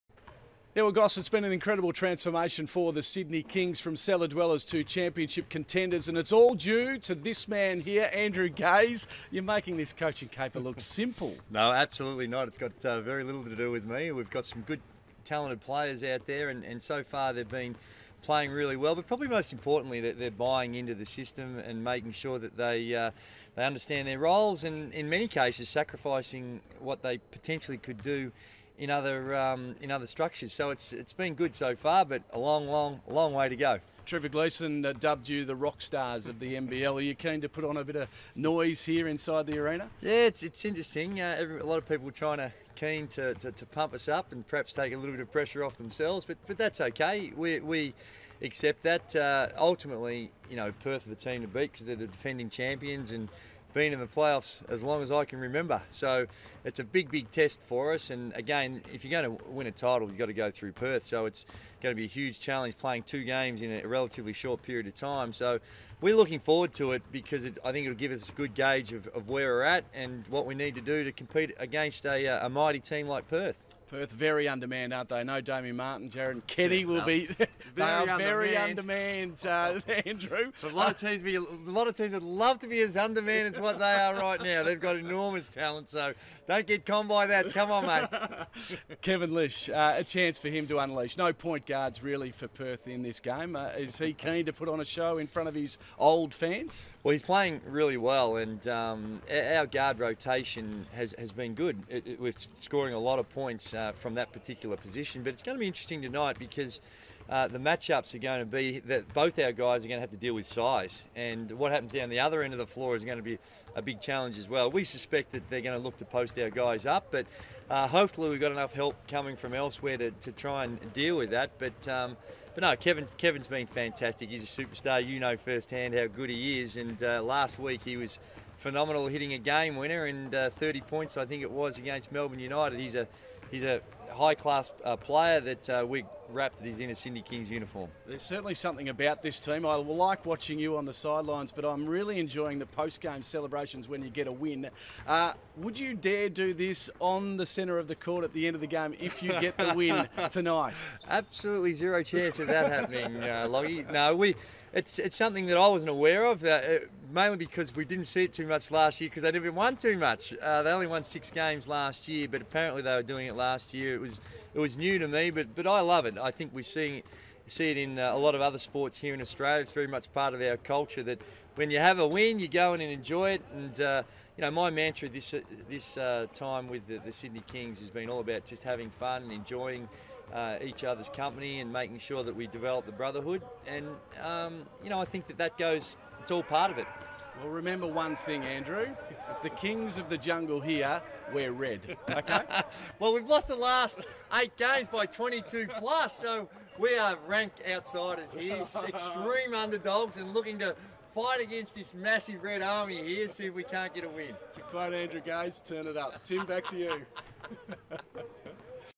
Lachy Reid interviews Andrew Gaze
Interview with Gaze ahead of Sydney Kings v Perth Wildcats clash